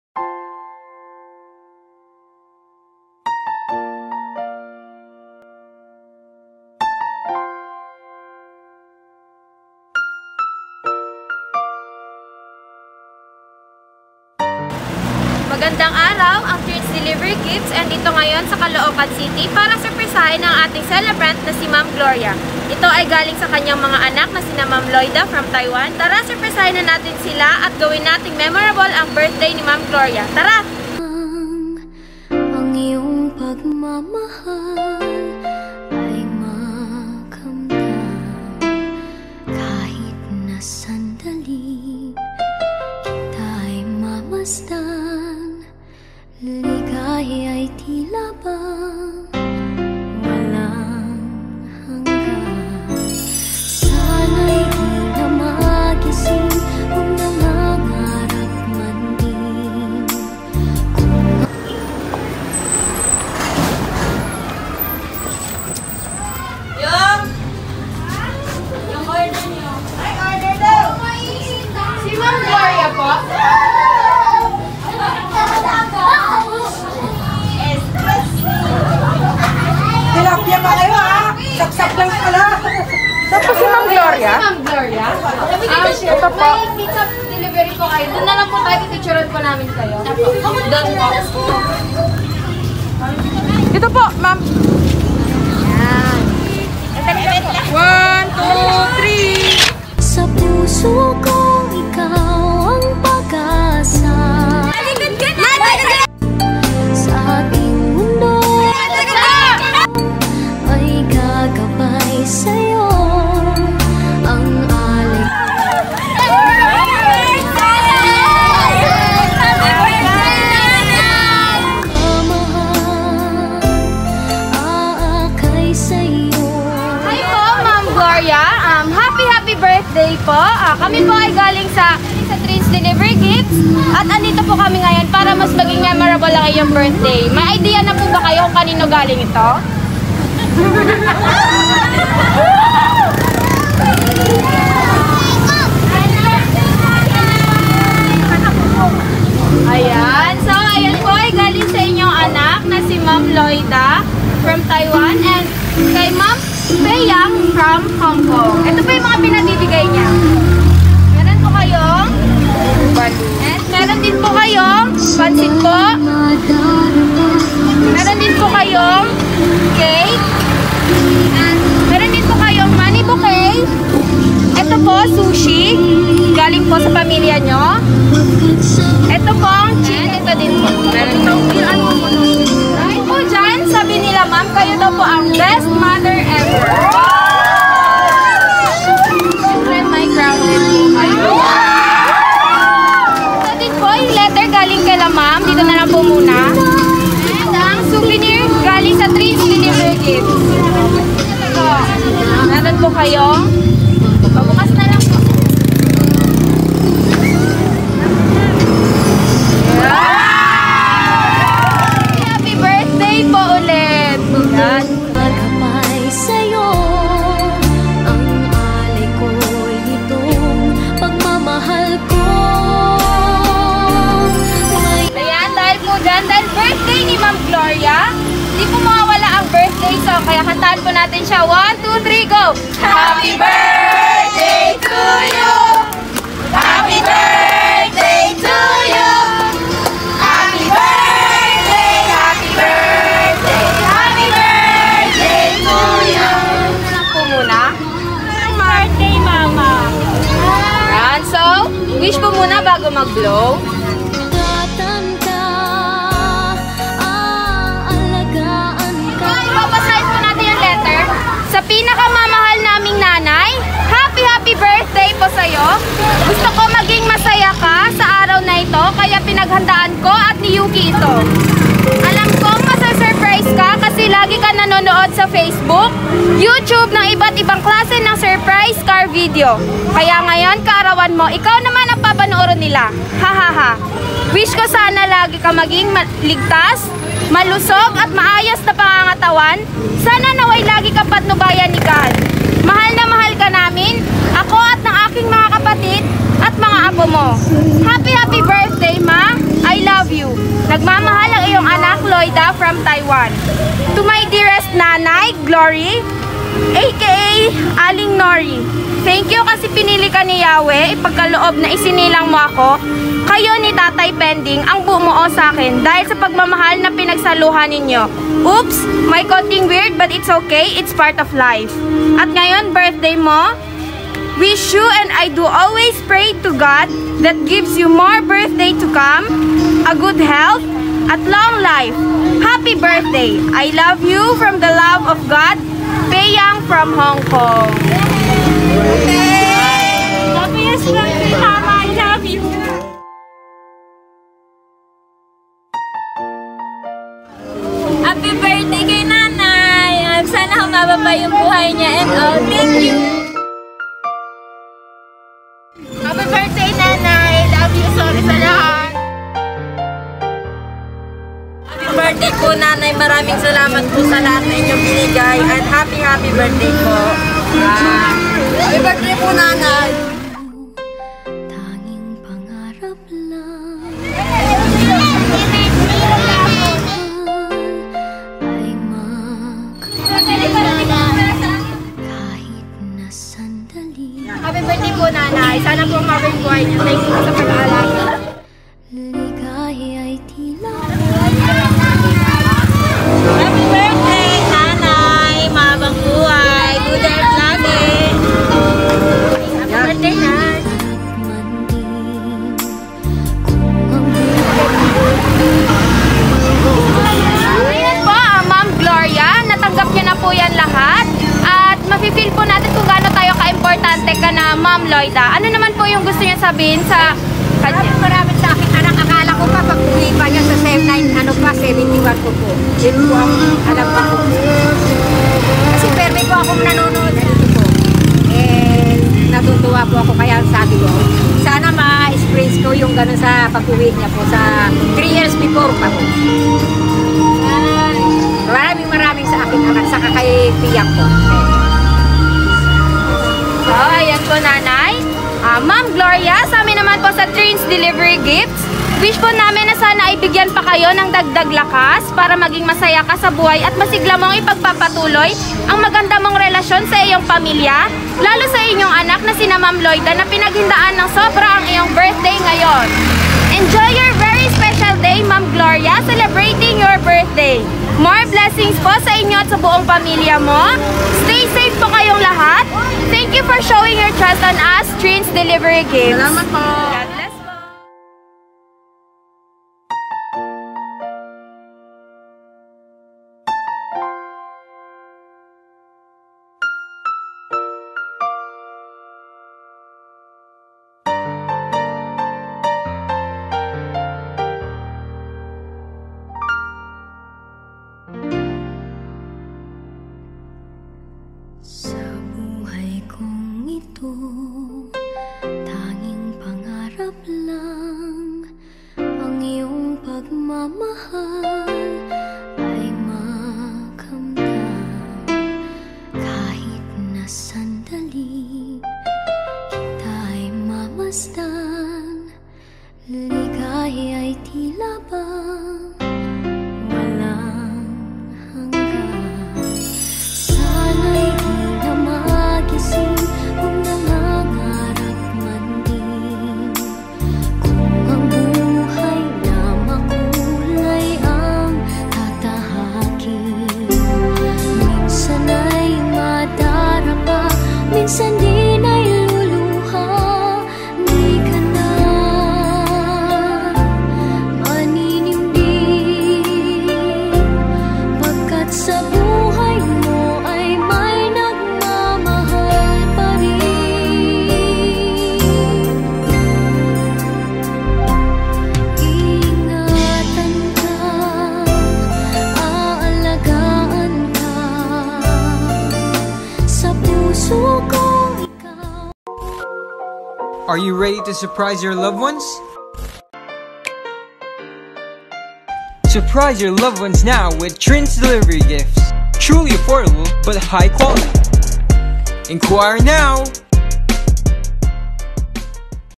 A SURPRIZE 68TH BIRTHDAY OF OUR NANAY GLORIA FROM TAIWAN WITH LOVE LOIDA
special thank to yuki ate daisy,peyang,william,jr,mga manugang at mga apo at sa mga nag effort sa pag prepare ng decoration and creativity from loida APRIL 4 2021 at 1pm